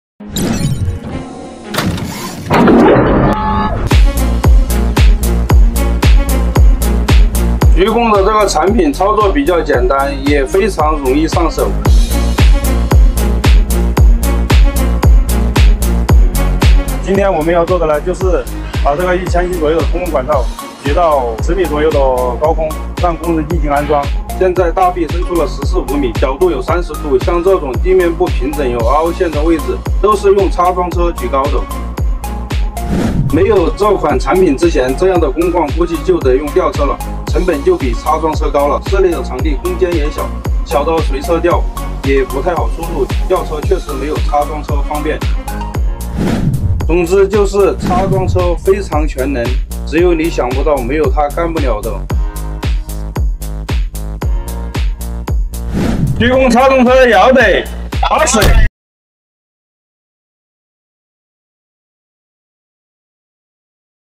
XCMG Telehandler construction record| Secondhand Machines| Construction machinery
XCMG's new generation K series telescopic forklift loader is applied with allnew visual identily appearance, panoramic cab, and optimized ergonomicdesign to realize high fashion, beauty, and operaion comfor. With electronic control gearshift,multiple steering modes, and frame hydraulic levelingtechnology, this product can be instaled with diversifed ateachments to reaize powerul working condition adaptability. The SAR rolover protection andsafety interlock control technology guarantees the operation safety